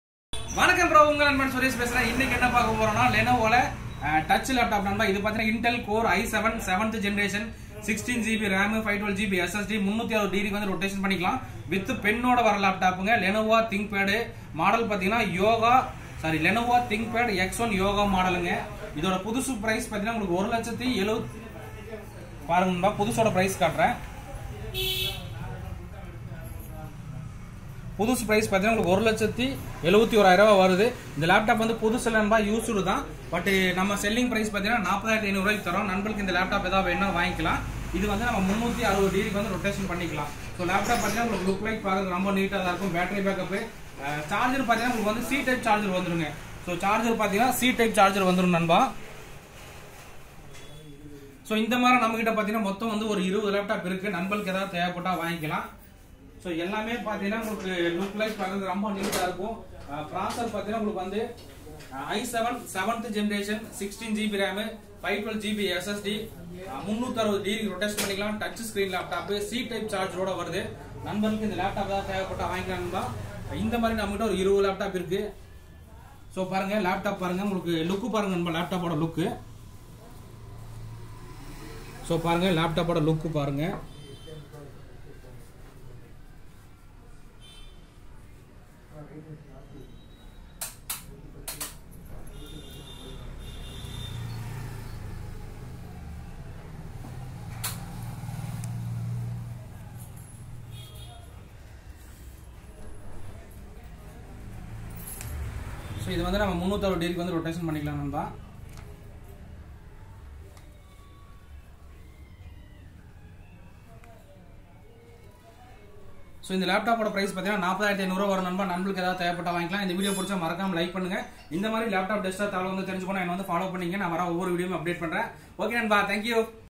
LENOVO IDEAPAD X1 YOGA TOUCH DISPLAY i7-7 GENERATION (20 NUMBER AVAILABLE)
NEW ONE PRICE Rs.1,70,000
OUR SELLING PRICE RS.40,500
LENOVO THINKPAD X1-YOGA. MODEL 360 ROTATION TOUCH
INTEL CORE i7-7 GENERATION
RAM 16 GB DDR4
512 GB SSD
360 DEGREE ROTATION
FULL HD TOUCH DISPLAY
WITH PEN
2 GB INTEL HD GRAPHICS CARD
WIFI
WEB CAMERA
BLUETOOTH
C- TYPE USB
C- TYPE ORIGINAL CHARGER
GOOD BATTERY BACKUP

20 NUMBER AVAILABLE

PRICE Rs.40500

OFFICE LOCATION MAP AND REVIEW:-

MERIN SYSTEMS

159, 5th St, Cross Cut Road, Gandhipuram, Tamil Nadu 641012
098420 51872